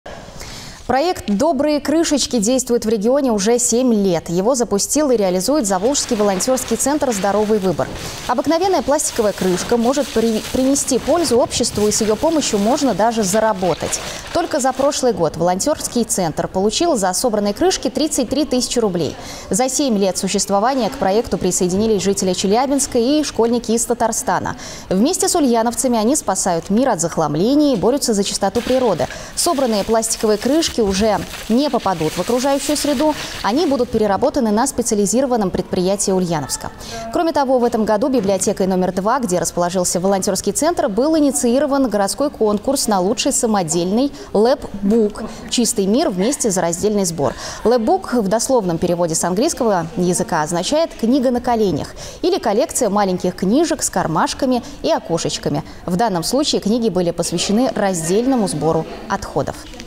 Проект "Добрые крышечки" действует в регионе уже 7 лет
Обыкновенная пластиковая крышка может принести пользу обществу и с её помощью можно даже заработать. Только за прошлый год волонтерский Центр получил за собранные крышки  33 тысячи рублей.